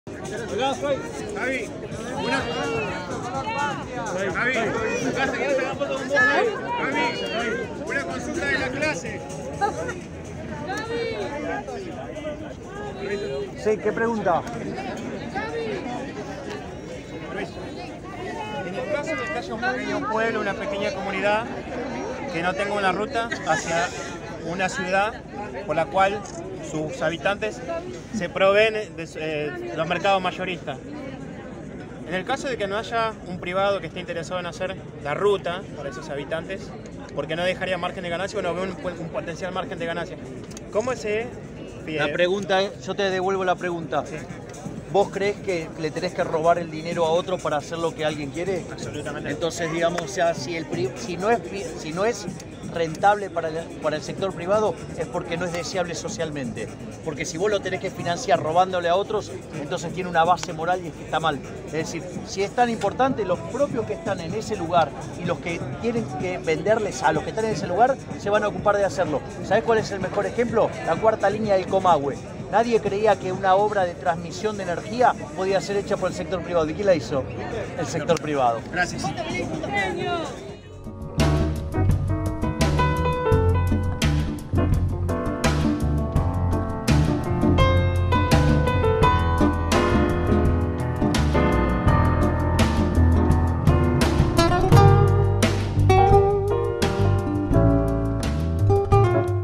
Milei recibe una consulta después de su clase sobre obra pública
Luego de dar la tercera de seis clases que está realizando en las distintas plazas de la ciudad, Javier Milei se acercó a saludar a sus seguidores y uno de ellos le realizó una consulta sobre la clase.